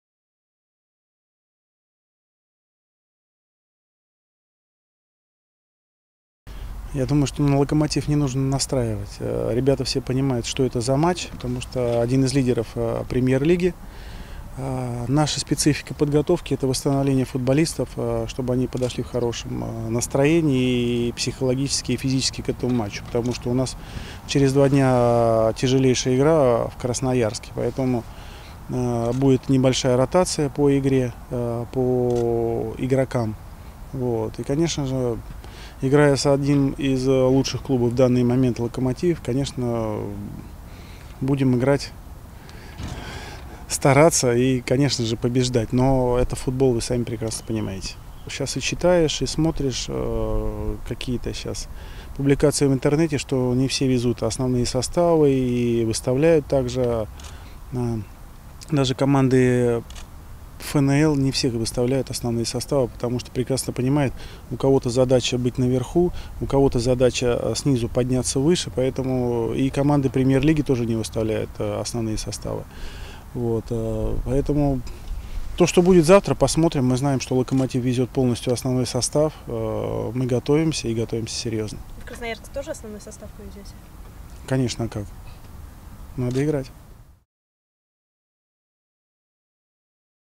Андрей Тихонов: «Ребята понимают, какой матч ждет нас с «Локомотивом»
В преддверии кубковой игры с "Локомотивом" главный тренер нашей команды Андрей Тихонов поделился ожиданиями от этой встречи.